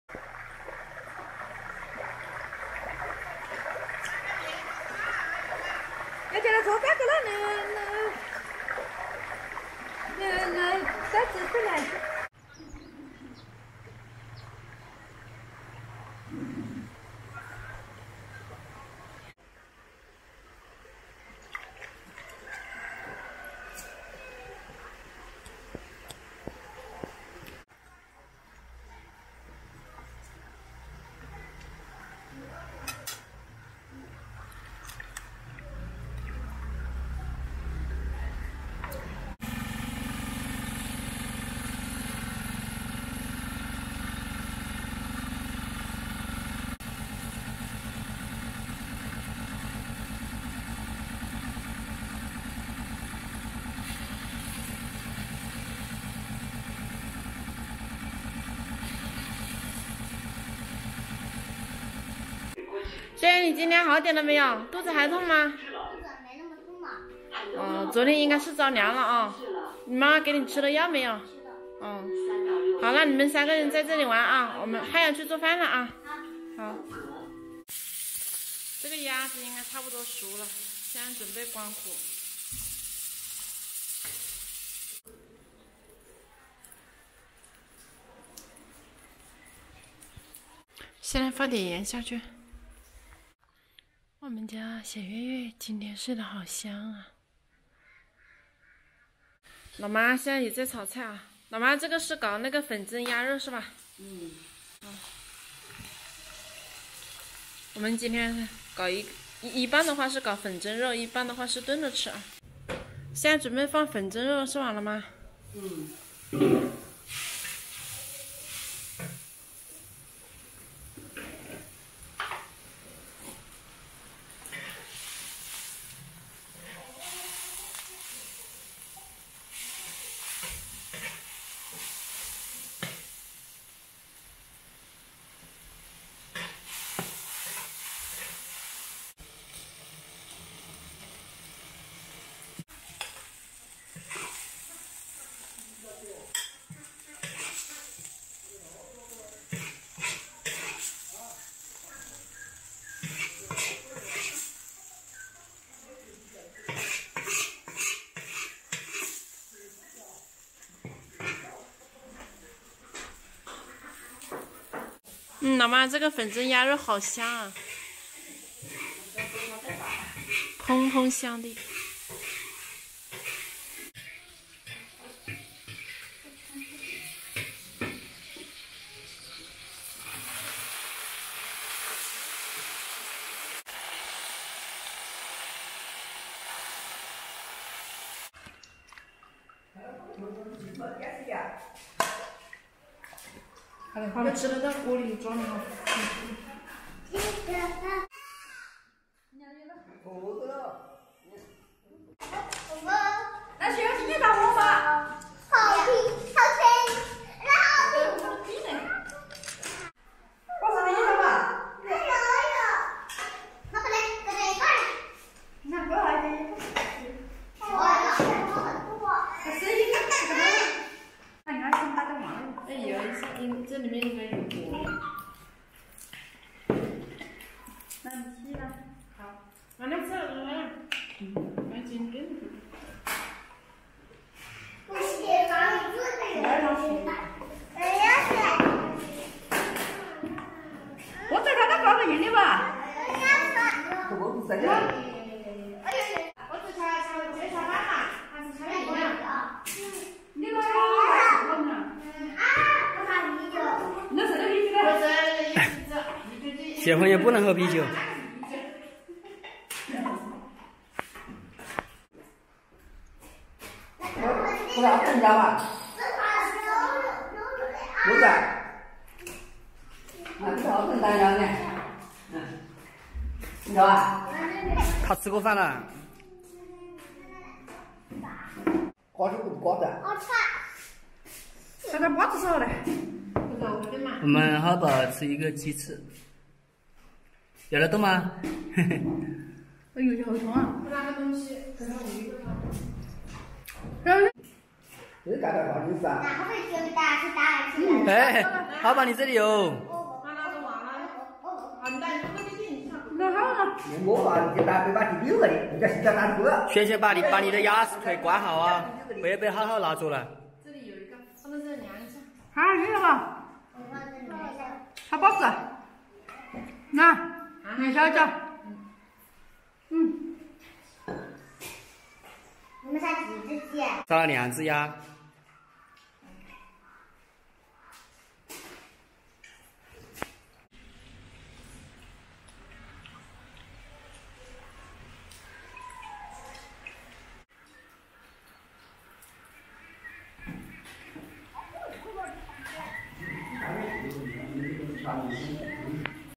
【ENG SUB】秋收季到了：婆婆殺兩隻土鴨，燉一半炒一半，兩個孩子搶著吃鴨肉【鄉野小希】
小白和爸爸在田里幹活，小希在家照顧孩子，媽媽在廚房做飯 ，一大家子人圍著餐桌吃飯，真熱鬧 。